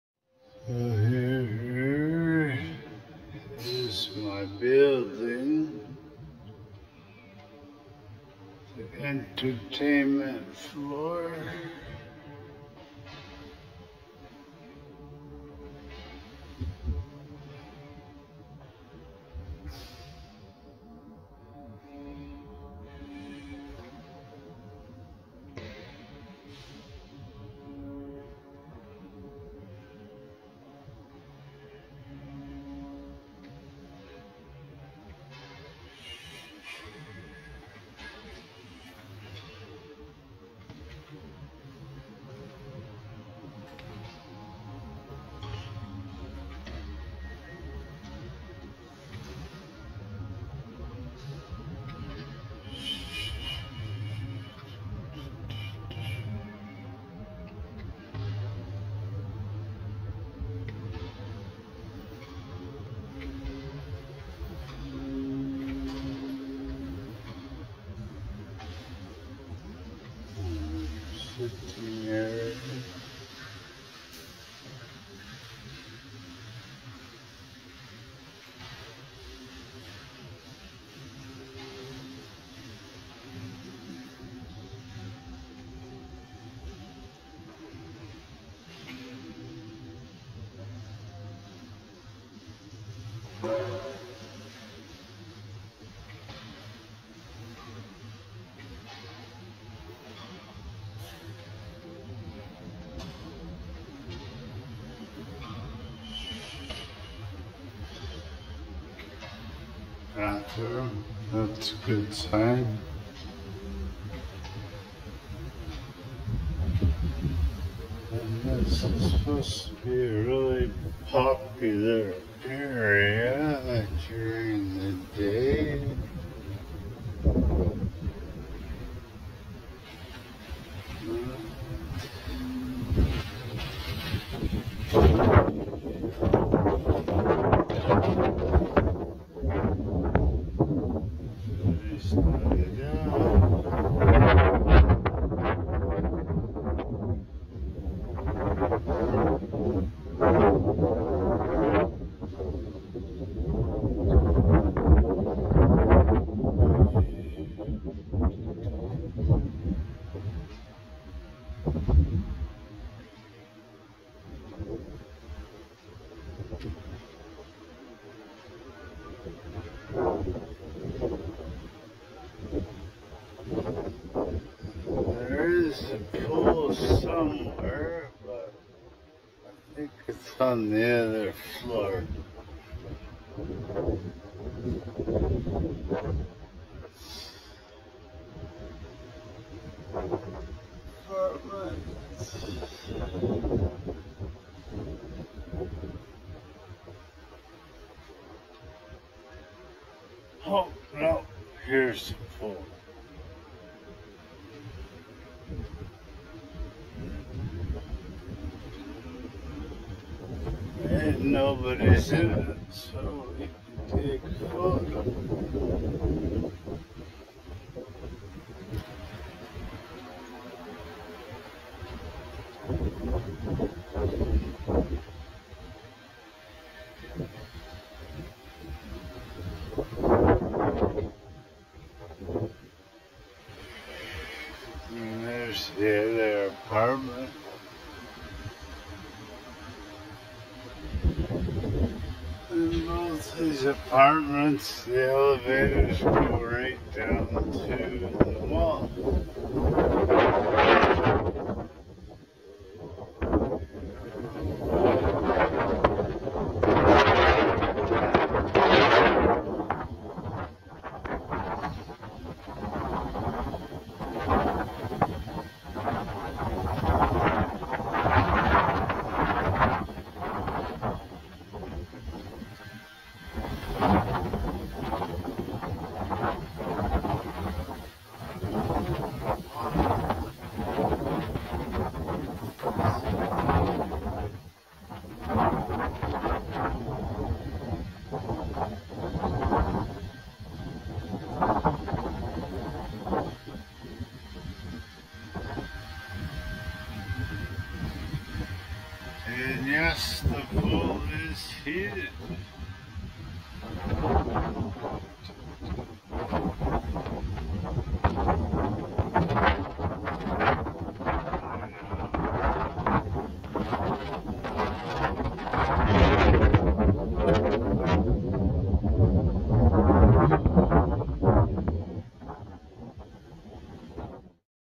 Airport Rental Apartments
Pradera rental apartments Tower 1, great place with lots of shopping and things to do.
close to the Airport for quick and east access, gym, pool as well, games rooms and sitting areas.